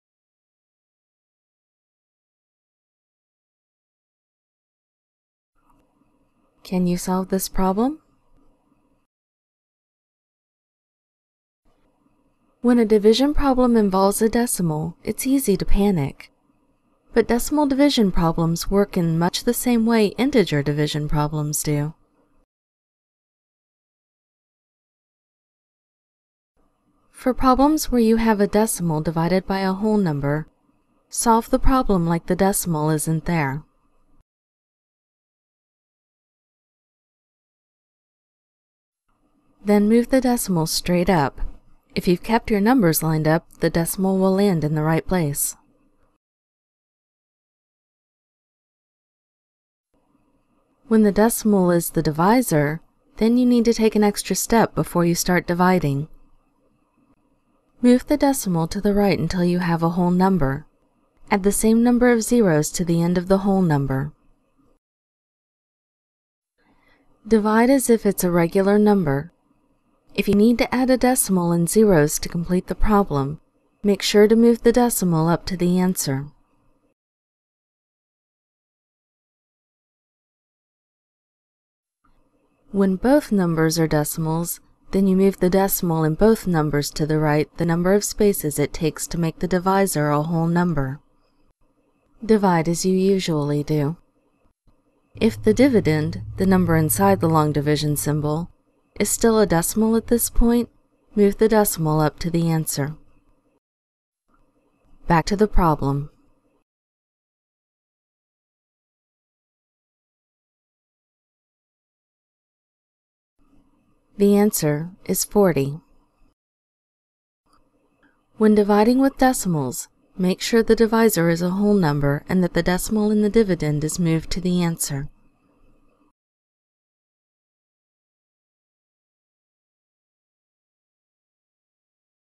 Dead Bunny's Guide to Dividing Decimals
When decimals are involved, division gets slightly more complicated. But when you know how to tackle the tricky step, the rest is pretty familiar.